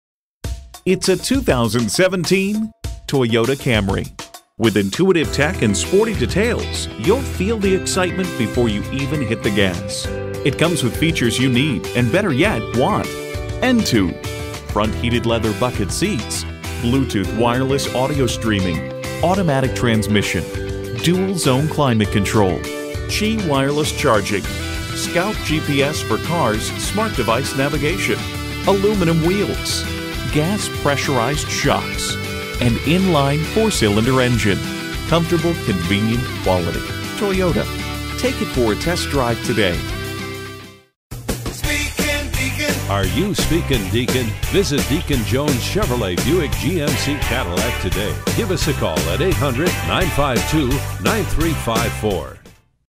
2017 Toyota Camry Smithfield NC Selma, NC GC0999 - SOLD
Year: 2017
Make: Toyota
Model: Camry
Trim: SE
Engine: 4 2.5 L
Transmission: Automatic
Color: Blizzard Pearl
Interior: Ash
Mileage: 106207
Stock #: GC0999
VIN: 4T1BF1FK7HU393968

Address:
1115 North Bright Leaf Blvd